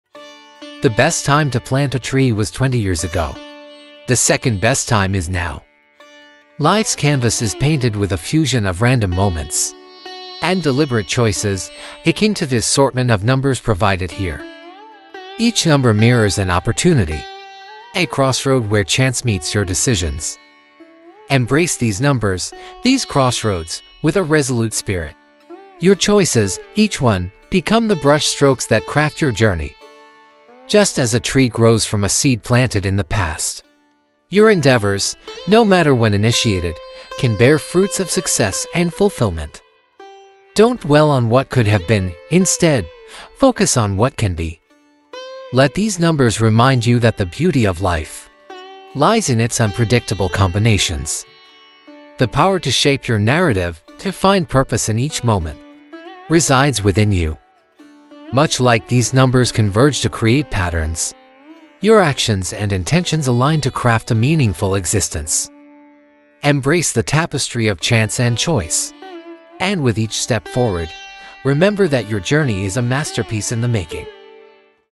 Embrace the Numbers: Crafting Your Unique Journey
Discover the profound connection between life's random numbers and the choices you make. Explore how each number represents an opportunity to shape your path, and learn to embrace the present with determination and purpose.